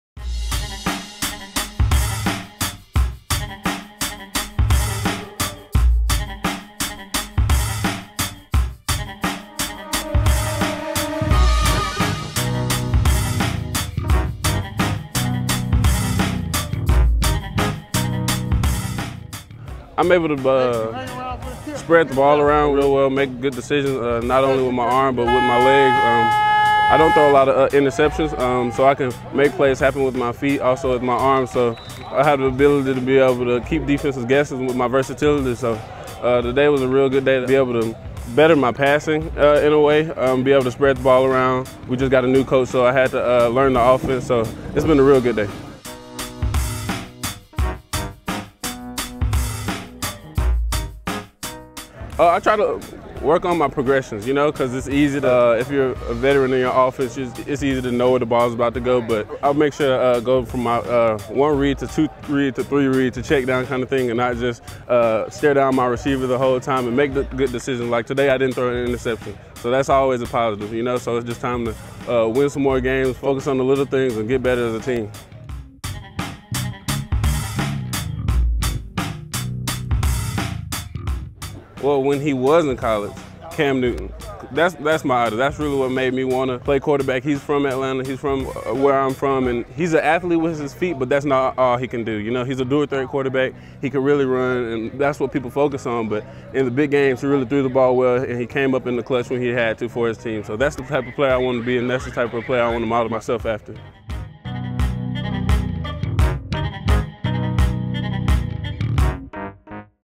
Highlights of Walton QB Josh White at Georgia 7on7
Tune in Tuesday, July 22, at  2:40 PM EDT to see Football  All_star_events Highlights of Walton (GA) QB Josh White at the 2014 National Select 7 on 7 Georgia Qualifier from Lassiter High School in Marietta, Georgia.  For full, live and on demand games from players like this go to NFHSnetwork.com

Produced By All Star